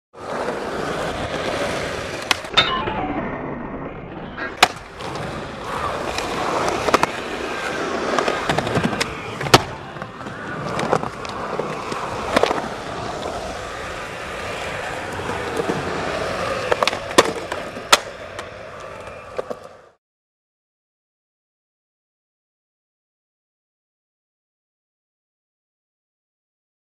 OTL #2
Just Angelo cruizing in the park like usual!